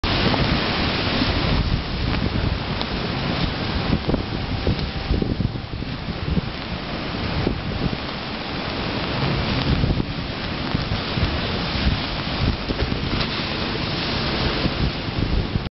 Great Western Railway in rough seas at Teignmouth, Devon, UK
High tide, very windy, so the waves were breaking onto the sea wall beside the railway line. This was filmed from a safe distance.  Earlier, I saw the line from a bridge above and could see that the waves were not breaking onto the railway. It was safe for the train to pass, but it looks spectacular.